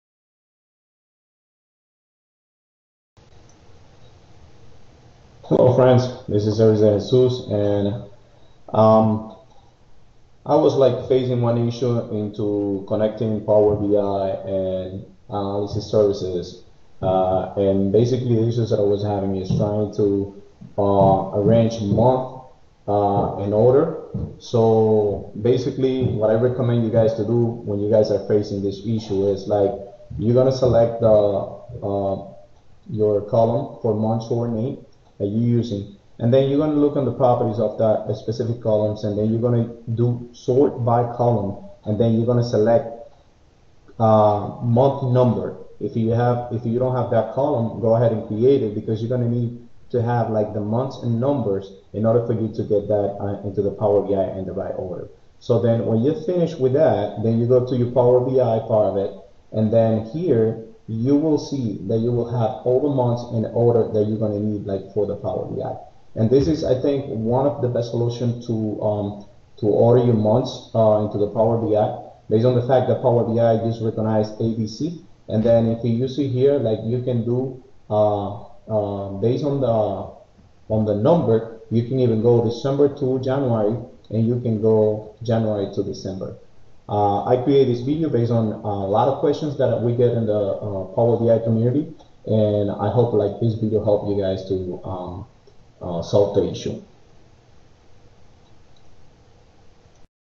How to sort month name in Power BI while using direct query
How to sort month name from Jan to Dec in Power BI while using direct query